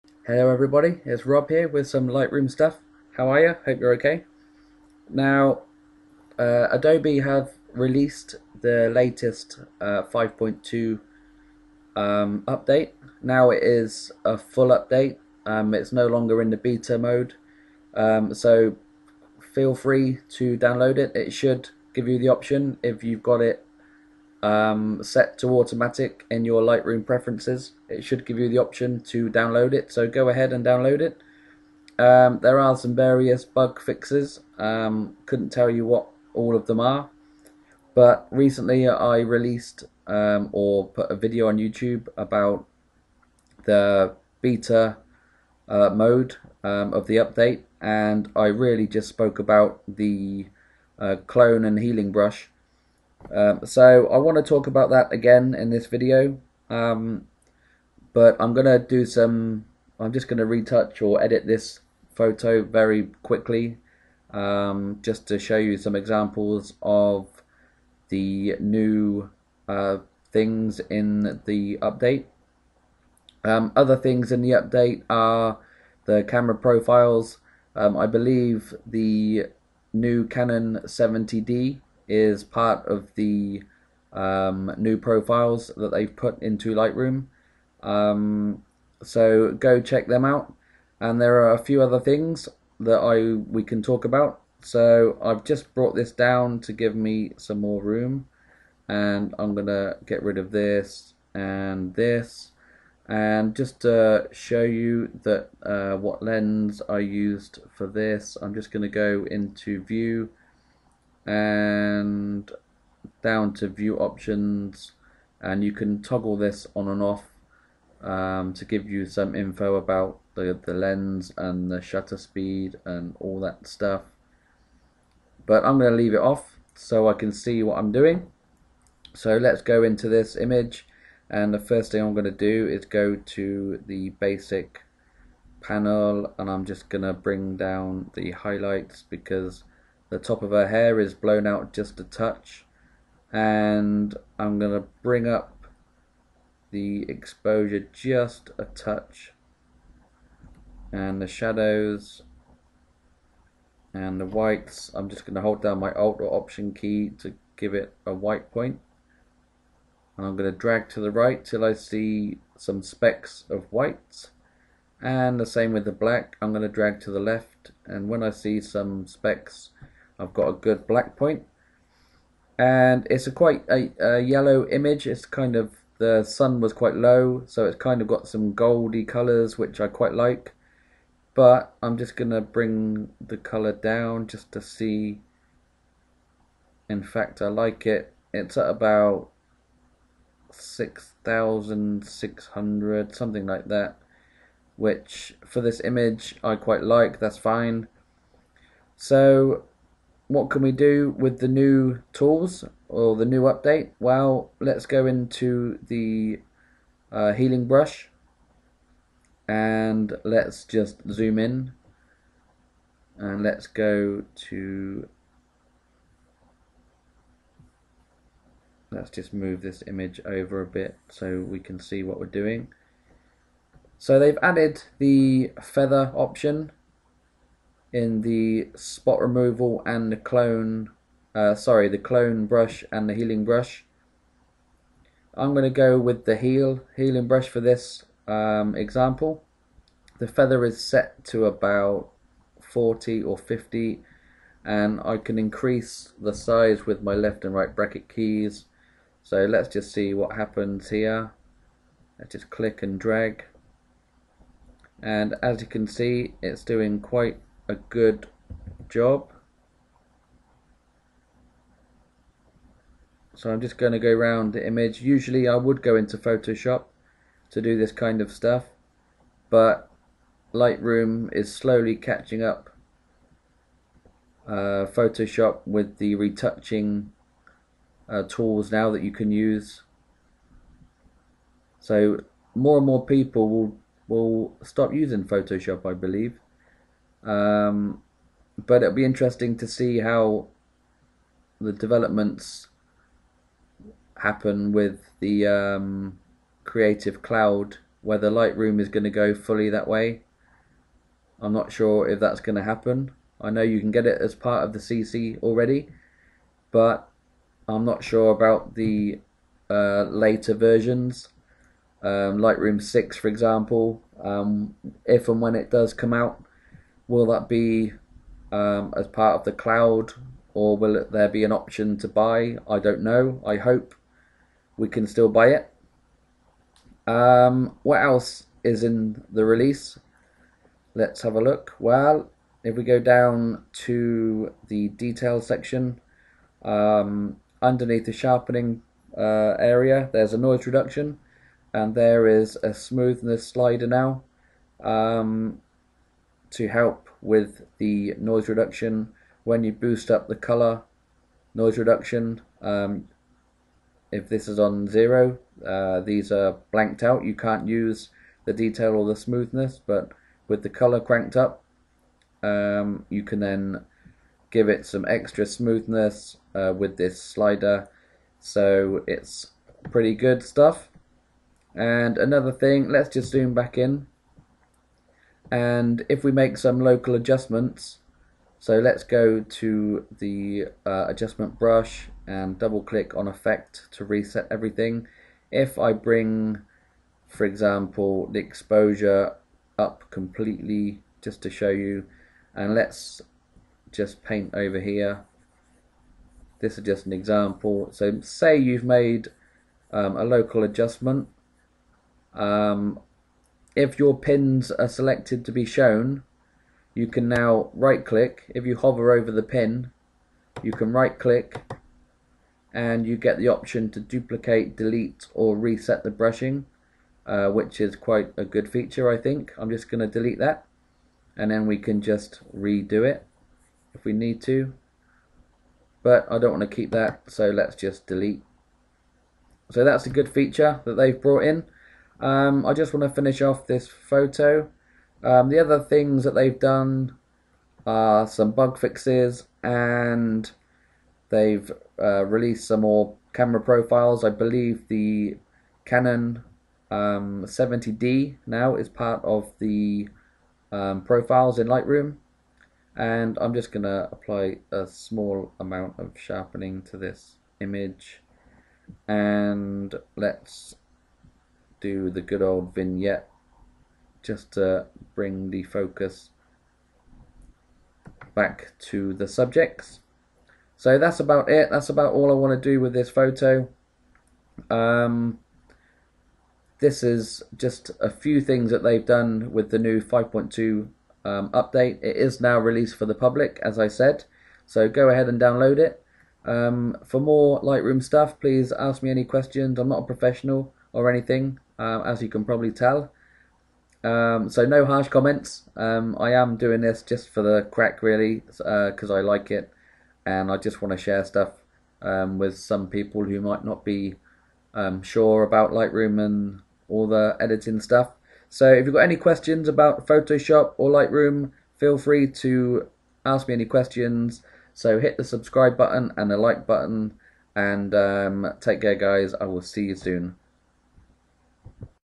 Lightroom 5.2 Update Released
Lightroom 5.2 !  Hello everyone.  Adobe have now released the update to the public.  There are bug fixes and some more camera profiles.  I believe the Canon 70D has been included in the new camera profiles.  The biggest change is the new feathering slider in the Clone/Healing brush, no more harsh edges and transitions.  I'm not a professional as you can tell lol, but I enjoy doing this so please no harsh comments.  Ask me any questions and I'll do my best to answer them.  Thanks and enjoy !